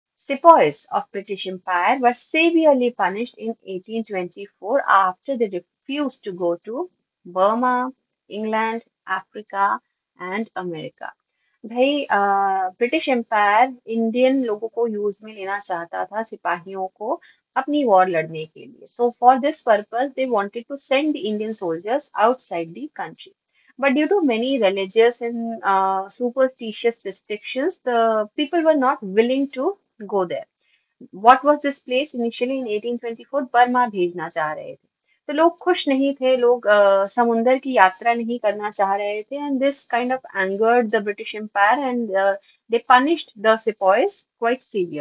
Sepoys of British empire were severely punished in 1824 after they refused to go to....
Sepoys of British empire were severely punished in 1824 after they refused to go to 📲PW App